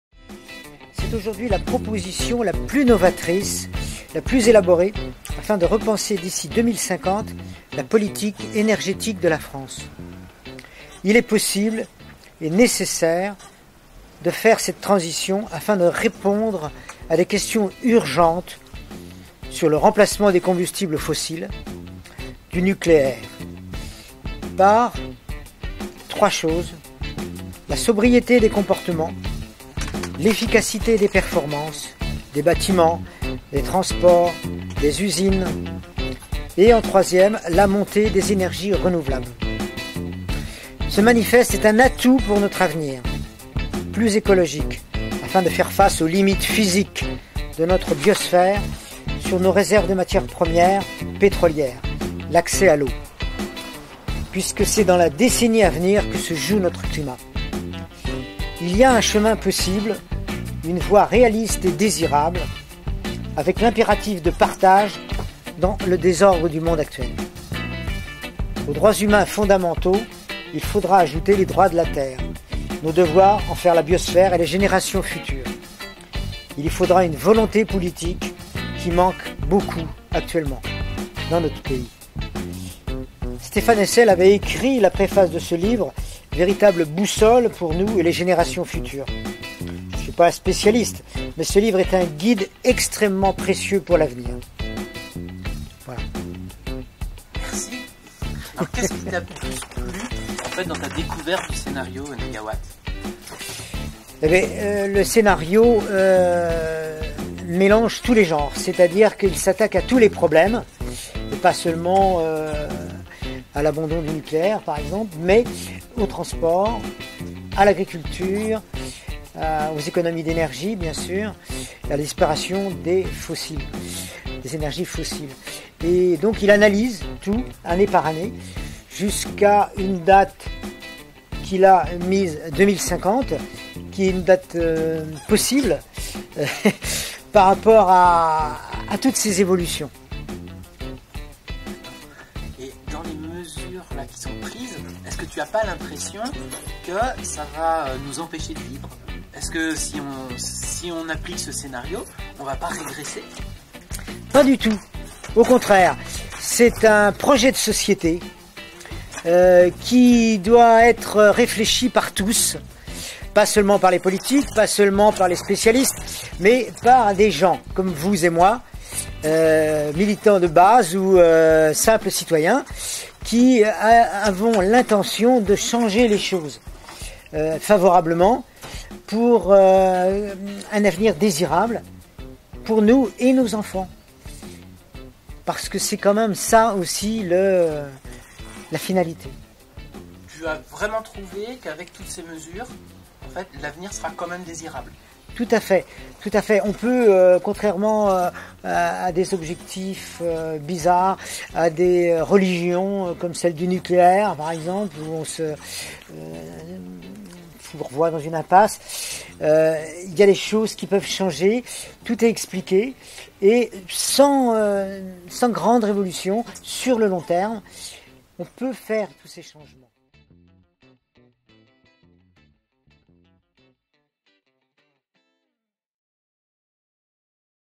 Les 11 et 18 juin 2017, je vote pour la transition énergétique
Le début d'une petite discussion entre écolos...